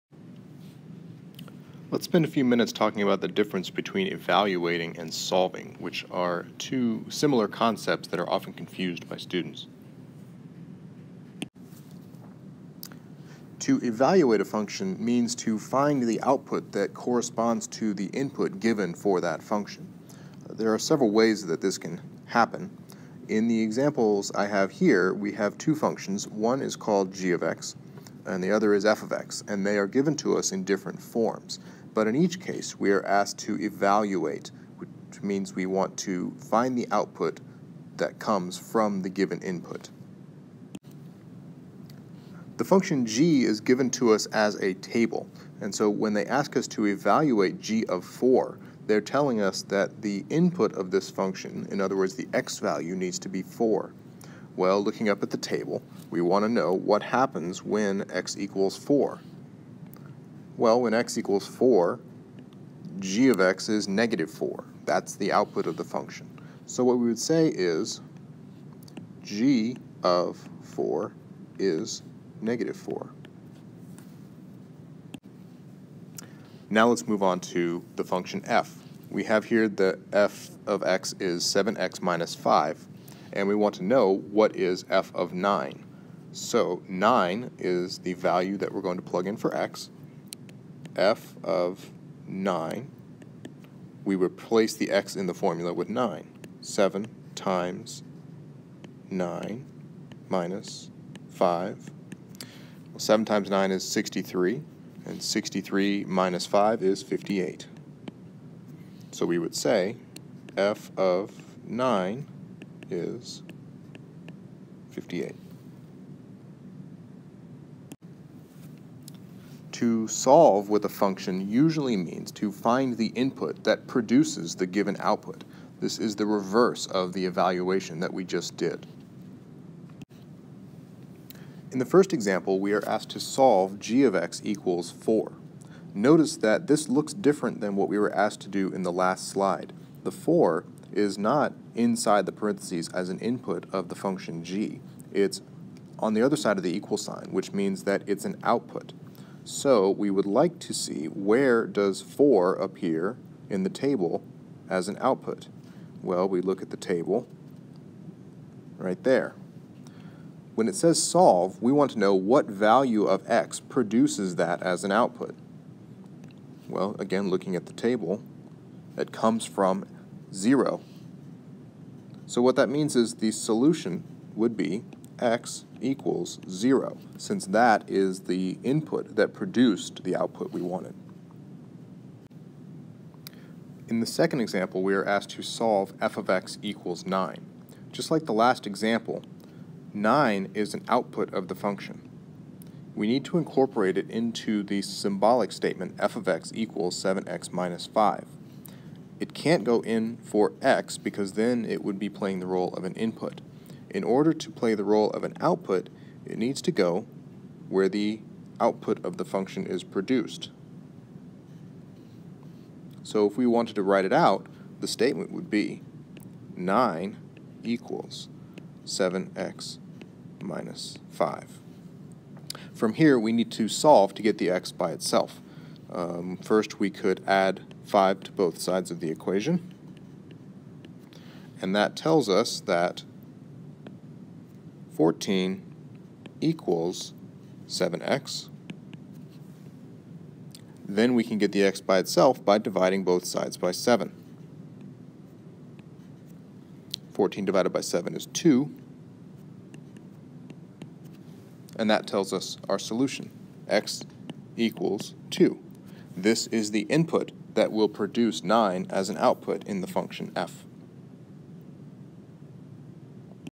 Video Assignment 2-3 Evaluating vs Solving
Examining the difference between evaluating and solving in algebra